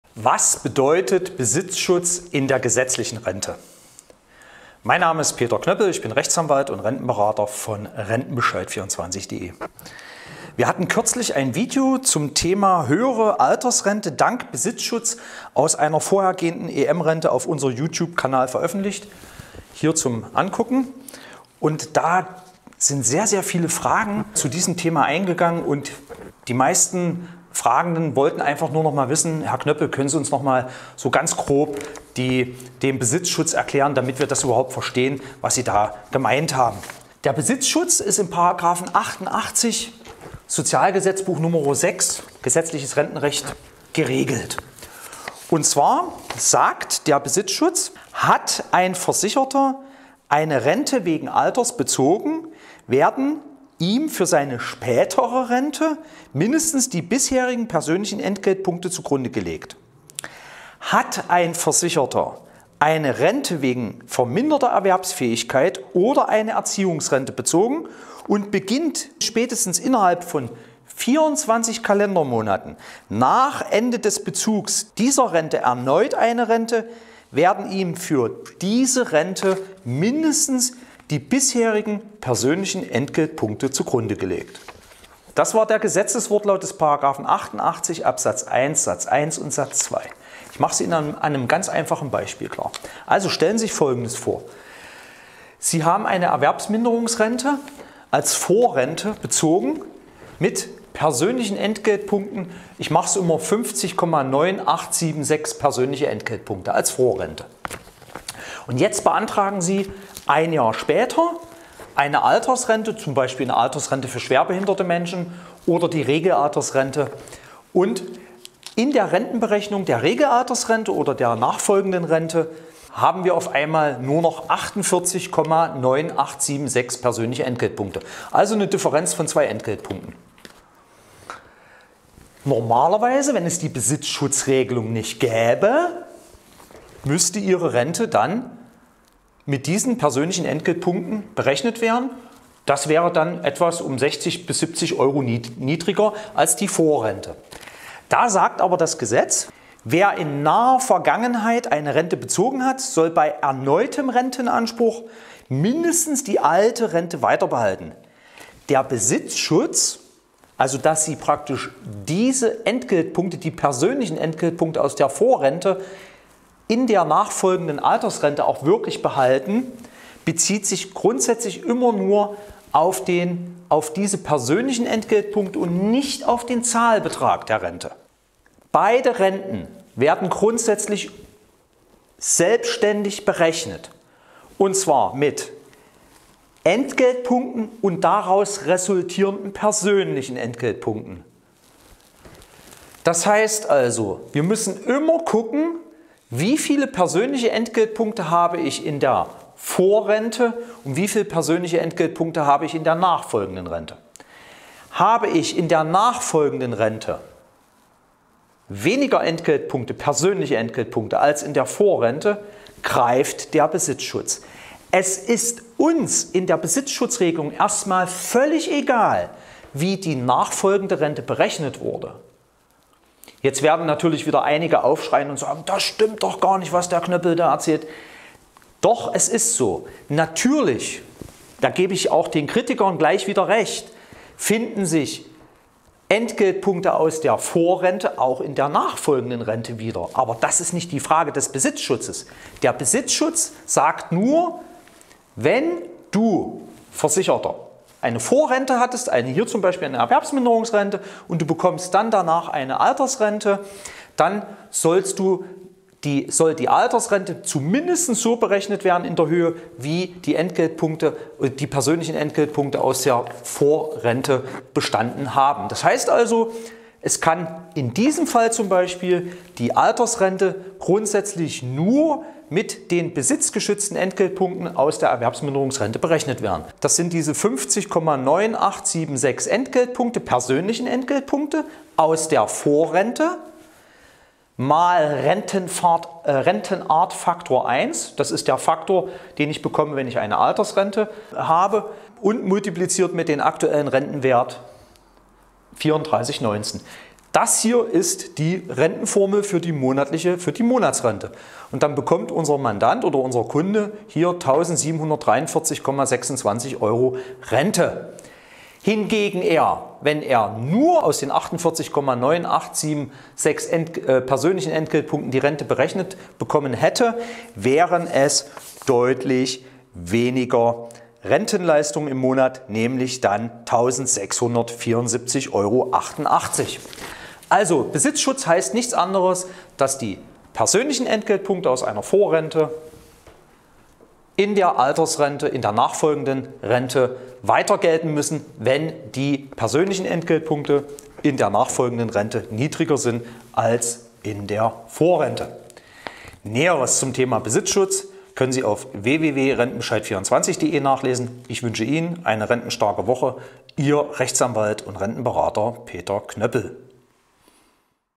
Höhere Rente durch besitzgeschützte Entgeltpunkte - Was genau ist das
Wie genau funktioniert der Besitzschutz eigentlich? Im §88 SGB VI stehen dazu die gesetzlichen Regelungen.